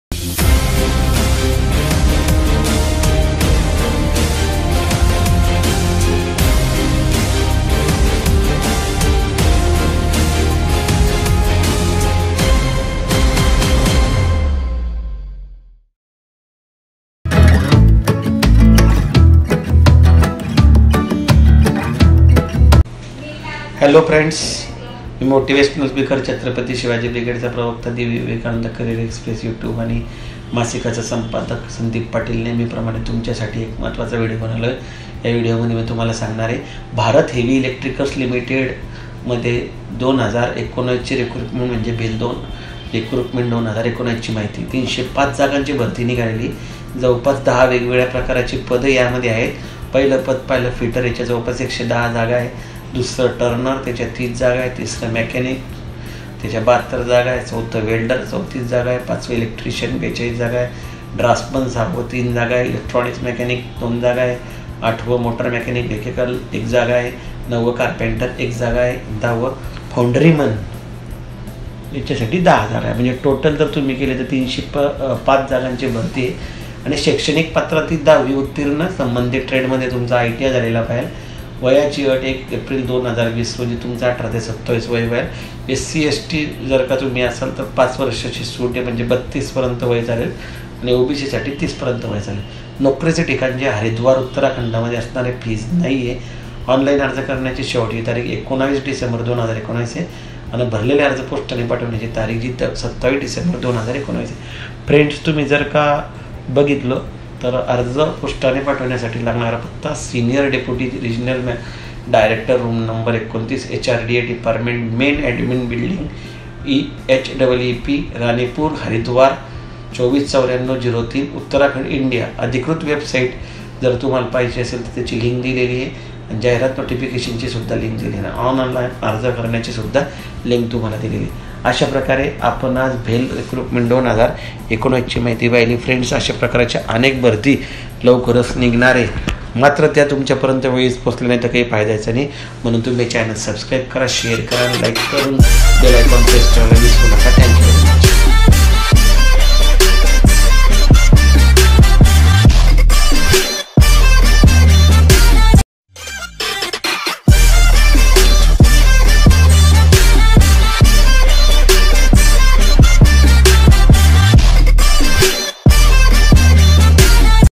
BHEL RECRUITMENT 2019
(BHEL) भारत हेवी इलेक्ट्रिकल्स लि. अप्रेंटिस पदांच्या 305 जागांसाठी भरती
Bharat Heavy Electricals Limited owned and founded by the Government of India, is an engineering and manufacturing company based in New Delhi, India. BHEL Recruitment 2019 (BHEL Bharti 2019) for 305 Apprentice Posts.
Total: 305 जागा
पदाचे नाव: अप्रेंटिस (प्रशिक्षणार्थी)
अ.क्र. ट्रेड पद संख्या
1 फिटर 110
2 टर्नर 30
3 मेकॅनिस्ट 72
4 वेल्डर 34
5 इलेक्ट्रिशिअन 42
6 ड्राफ्ट्समन (मेकॅनिक) 03
7 इलेक्ट्रॉनिक्स (मेकॅनिक) 02
8 मोटर मेकॅनिक वेहिकल 01
9 कारपेंटर 01
10 फॉउंडरी मन 10
 Total 305
शैक्षणिक पात्रता: (i) 10 वी उत्तीर्ण  (ii) संबंधित ट्रेड मध्ये ITI.
वयाची अट: 01 एप्रिल 2020 रोजी 18 ते 27 वर्षे.  [SC/ST: 05 वर्षे सूट, OBC: 03 वर्षे सूट]
नोकरी ठिकाण: हरिद्वार (उत्तराखंड)
Fee: फी नाही.
Online अर्ज करण्याची शेवटची तारीख: 19 डिसेंबर 2019
भरलेले अर्ज पोस्टाने पाठविण्याची शेवटची तारीख: 27 डिसेंबर 2019
अर्ज पोस्टाने पाठविण्याचा पत्ता: Senior Dy. Regional Director, Room No.-29, HRA Department, Main Admin Building HEEP, Ranipur, Haridwar-249403, Uttarakhand, India.